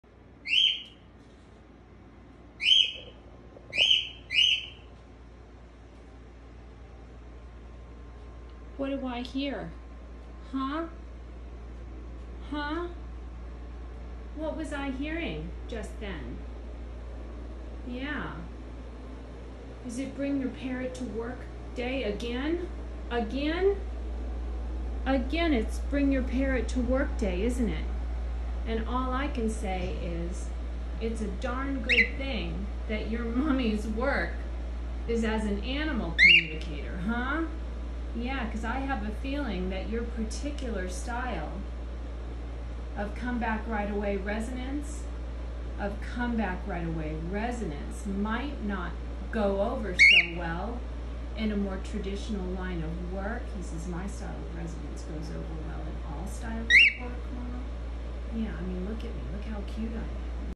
It is bring your parrot to work day again
It’s “bring your parrot to work day” again. 🙌🦜🤪
Yup. My office manager (heck, supervisor - who are we kidding;) has feathers. All over his cute feathery body. And a shriek that can stop traffic and send near neighbors running to close their open windows and doors. 🎤🚪🪟🦜
But you want to hear something amazing? As soon as I sit down to communicate with an animal, all I have to do is settle Pearl on my lap and say “we need to work now” and I don’t hear a peep out of him until the session ends! The animals know. They hear us. They feel our intentions and respond to our energy. They want to support us. Often, they just don’t know how because we have never made a clear request! 😯
Animal communication is real. I got my first tangible experience of it when I hired an animal communicator. Now I am able to pay it forward. 💕